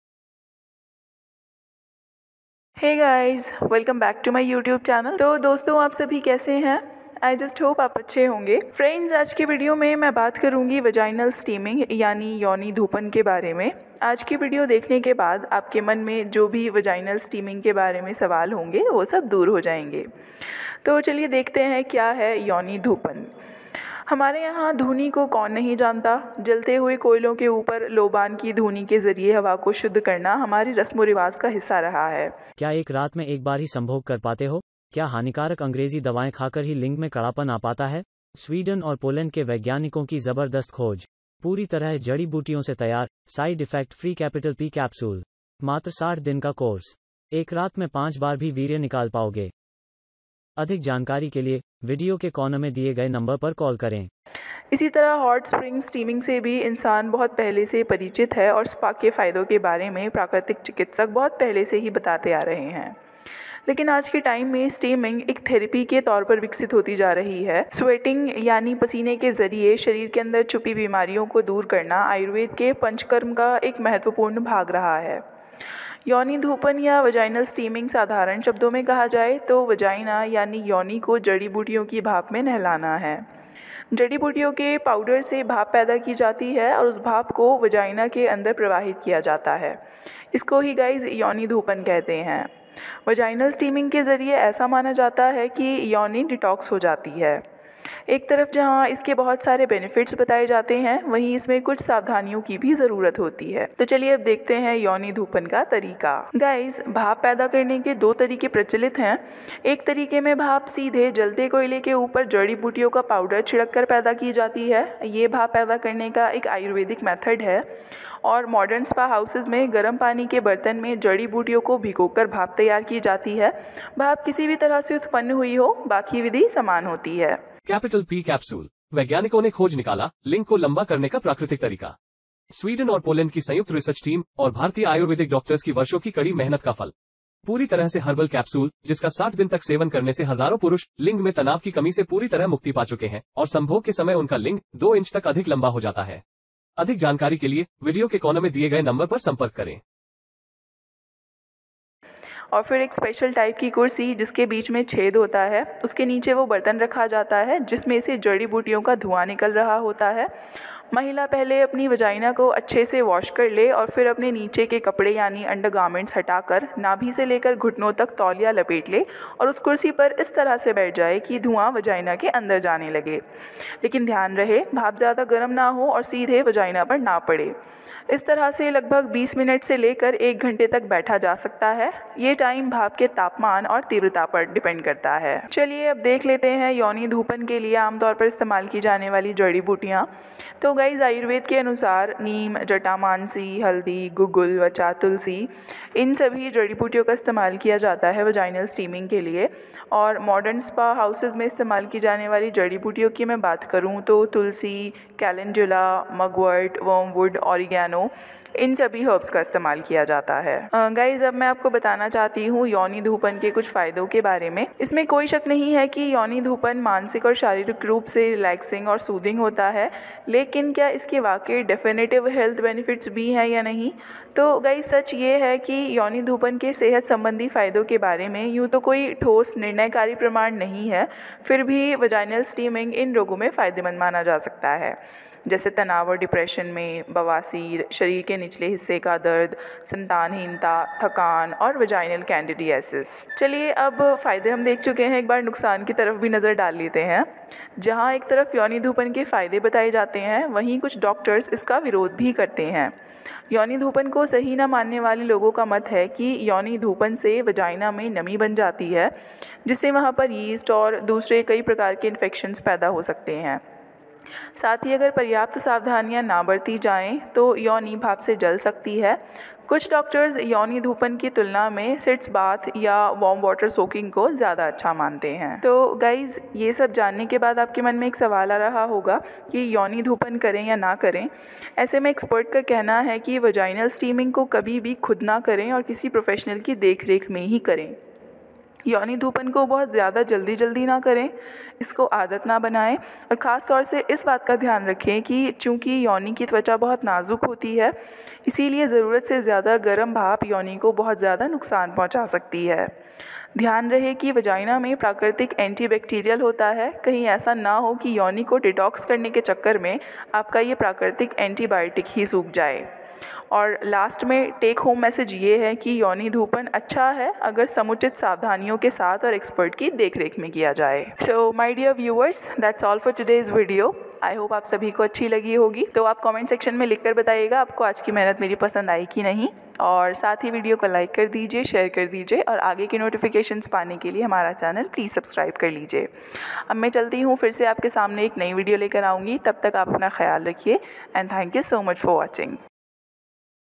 योनि धूपन क्या होता है, इसके फायदे और करने का तरीका । Dr. Nadiya Cindy
What is vaginal steaming and how to utilize this therapy to keep vagina healthy. Ayurveda ke anusar yoni dhupan ke fayde, pryog vidhi, aur iske nuksan. yonidhupan vaginalsteaming steambath Yoni dhupan me kaun si herbs use hoti hain. Herbs for yoni dhupan.